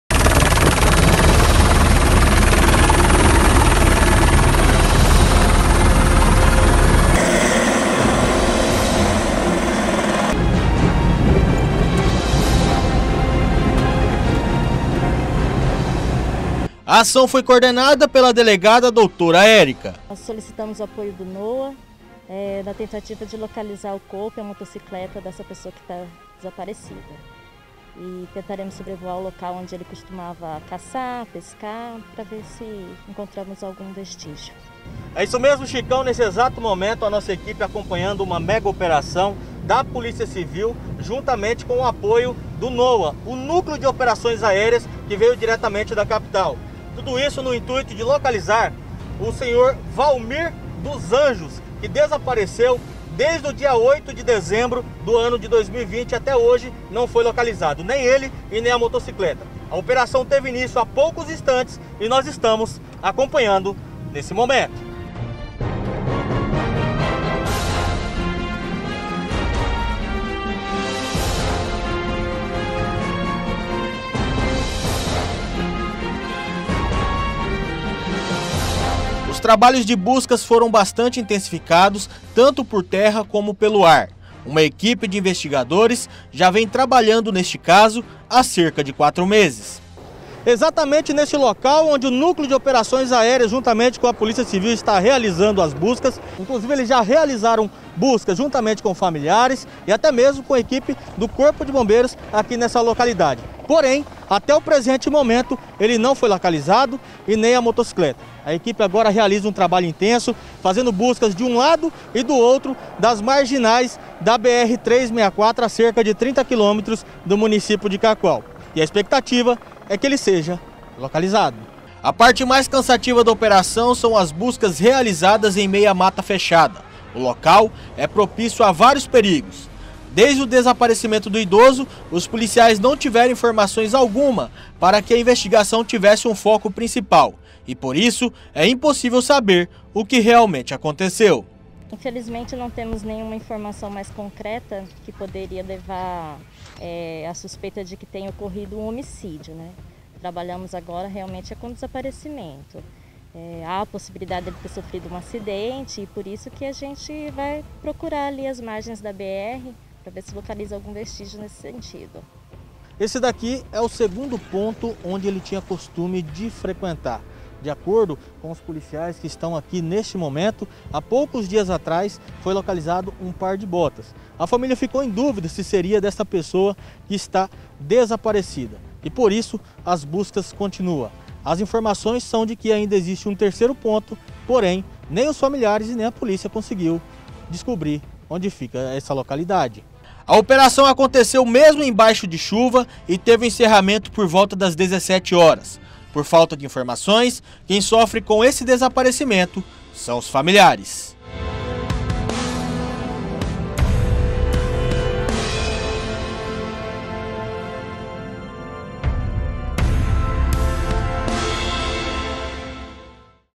OPERAÇAO VALMIR
POLÍCIA CIVIL E NOA REALIZAM BUSCAS DE HOMEM DESAPARECIDO DESDE DEZEMBRO DE 2020 EM CACOAL.//